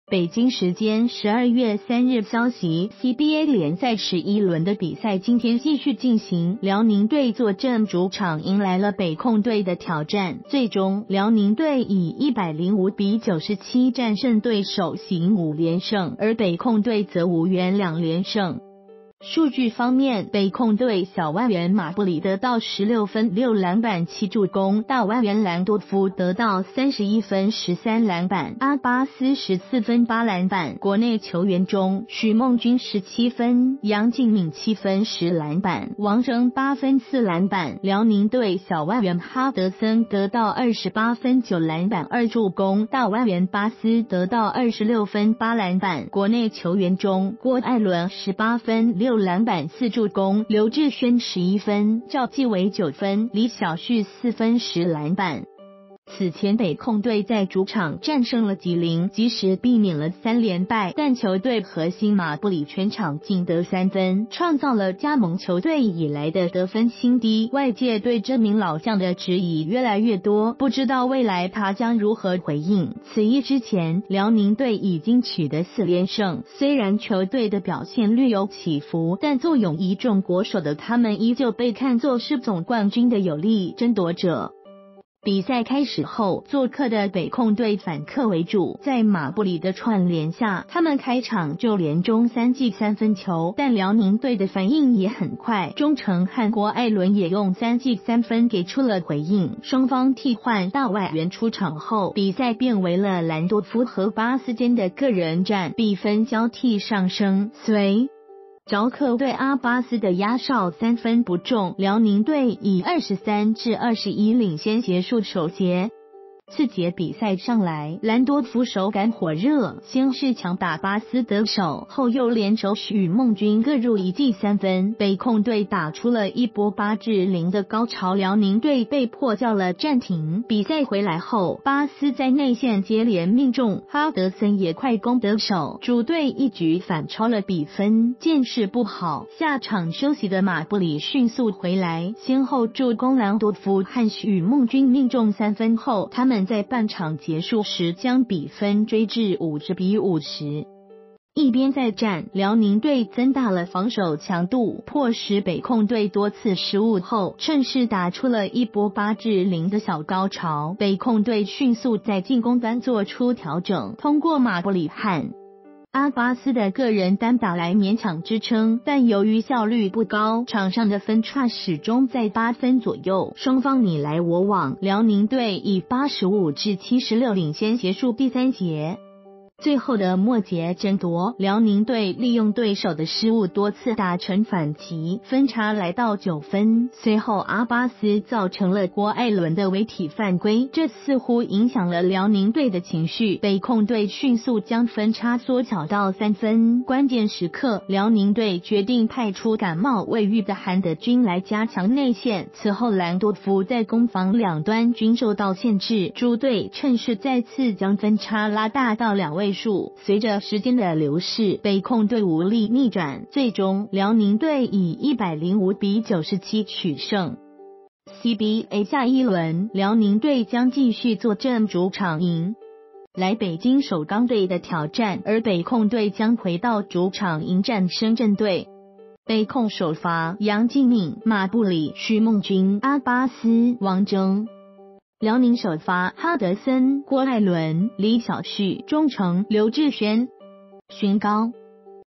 郭艾伦18分6篮板 辽宁主场大胜北控收获五连胜
發表評論評論如下。